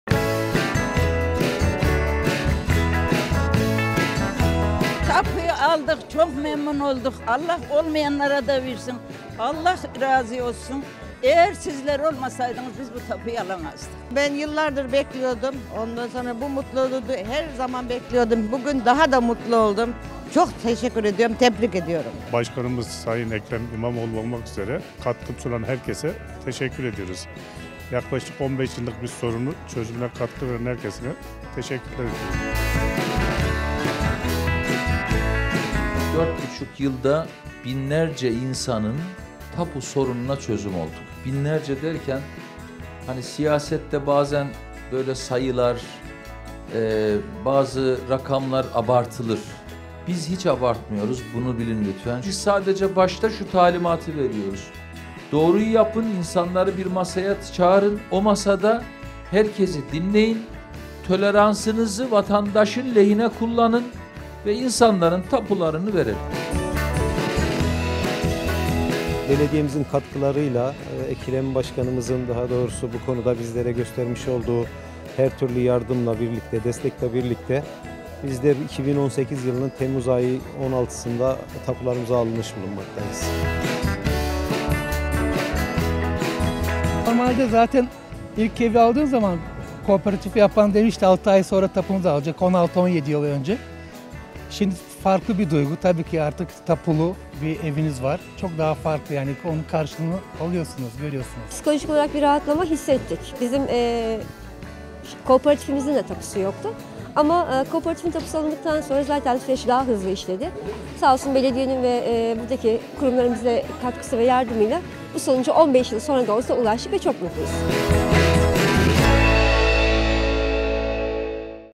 Tapu Sevinci
34 haneli Genç Kuranlar Yapı Kooperatifi 15 yıldır tapu sorunu yaşıyordu.

Beylikdüzü Belediyesi vatandaşların 15 yıllık tapu hasretine son verdi.

Barış Kafe’de düzenlenen törende Başkan İmamoğlu ev sahiplerine tapularını dağıttı.